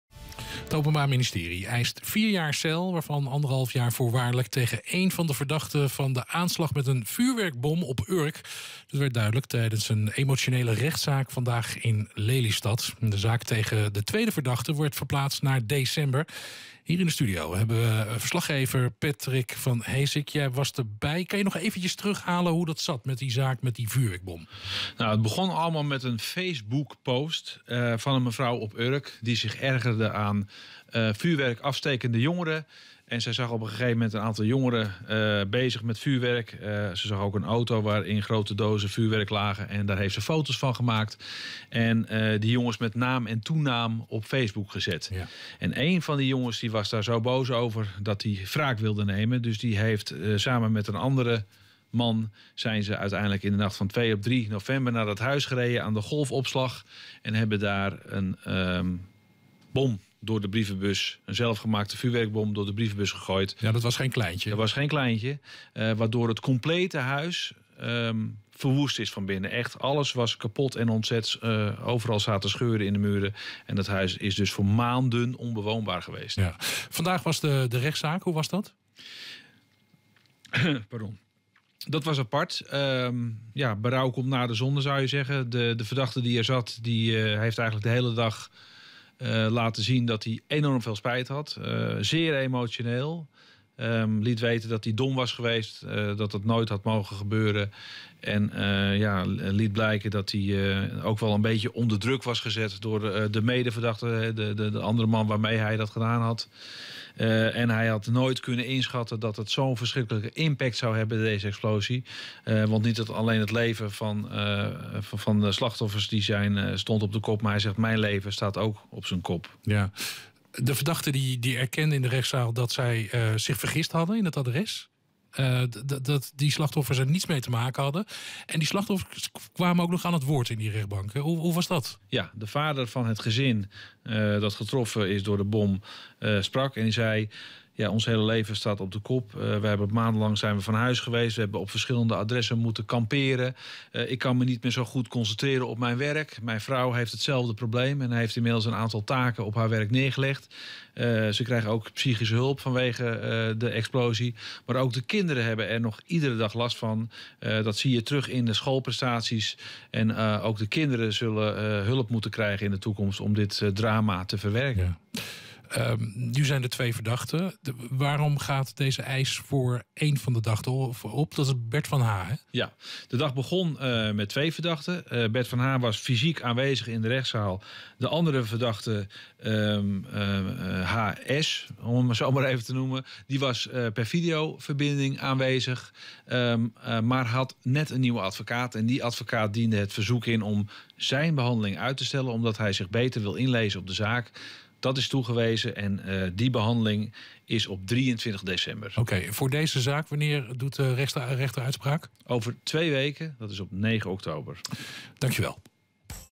Eis: 4 jaar cel voor Bert van H. in zaak vuurwerkbom Urk
Het Openbaar Ministerie eist vier jaar cel, waarvan 1,5 jaar voorwaardelijk tegen één van de verdachten, Bert van H., van de aanslag met een vuurwerkbom op Urk. Dat werd duidelijk tijdens een emotionele rechtszaak vrijdag in Lelystad. De zaak tegen een tweede verdachte werd verplaatst naar december.